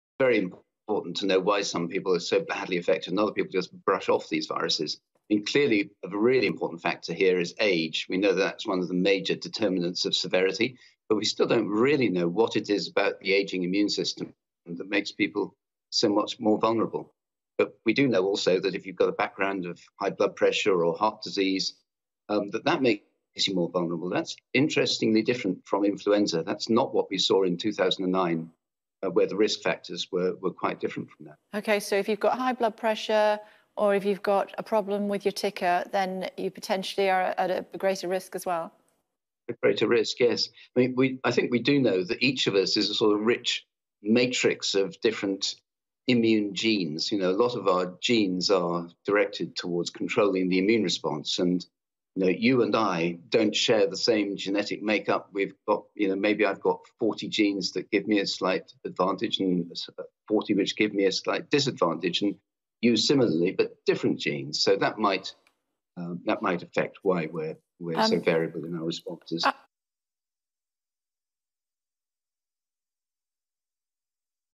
Coronavirus: Who's most at risk?
"A really important factor here is age."

Peter Openshaw, Professor of experimental medicine at Imperial College London, explains who is more at risk from COVID19.